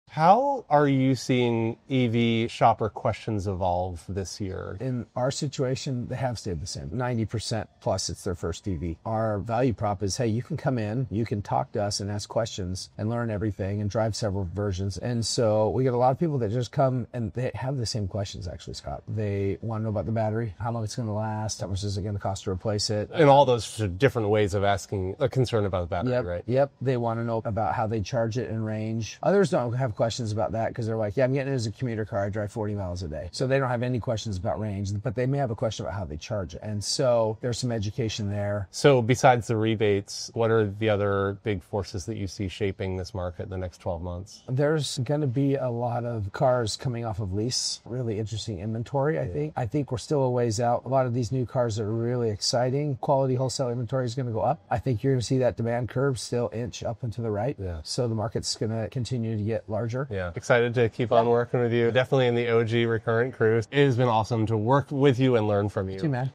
Talking EVs With The CEO of EV Auto 🤔
Two EV CEOs discuss the current EV landscape.